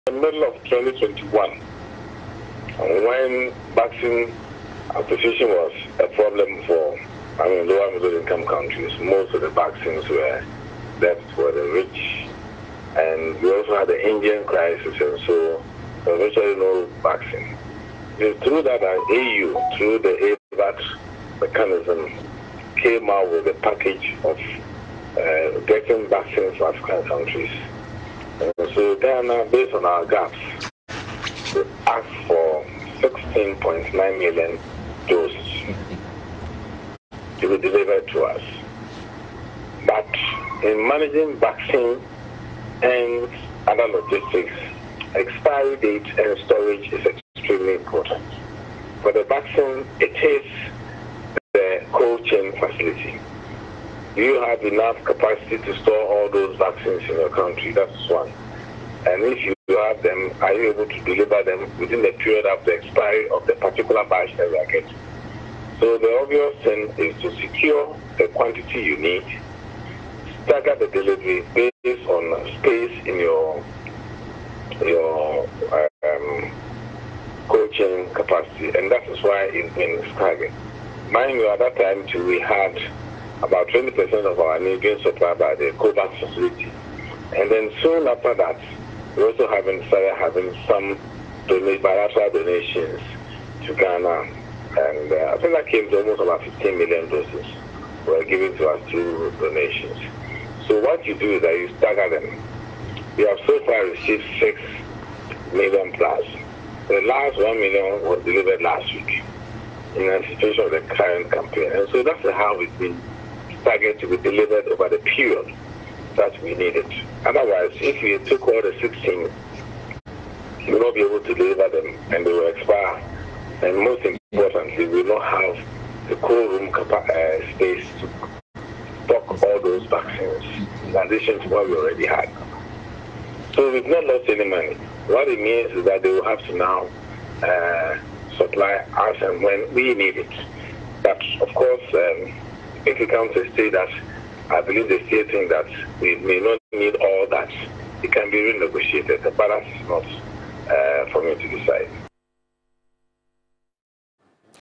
COVID 19 funds utilization.
Director General of the Ghana Health Service, Dr Patrick Kumah-Aboagye has been explaining why $81.8M worth of Covid19 vaccines were not delivered to the country as contained in the auditor general’s report.